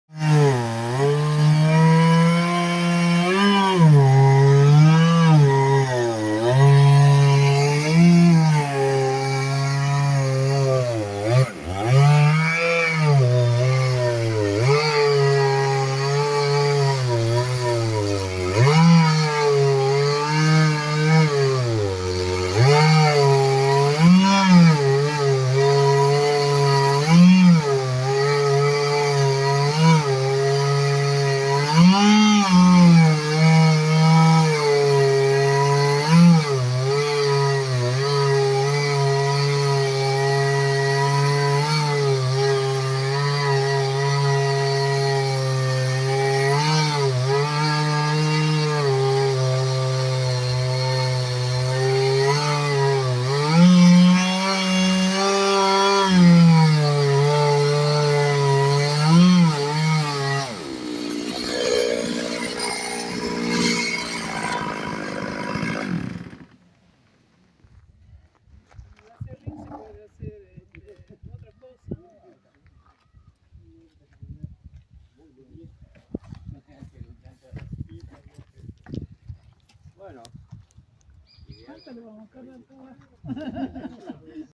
cortando tablas con motosierra y guia
Asi es como realizamos nuestras tablas de asado/muebles.

Here's how we make our barbecue tables/furniture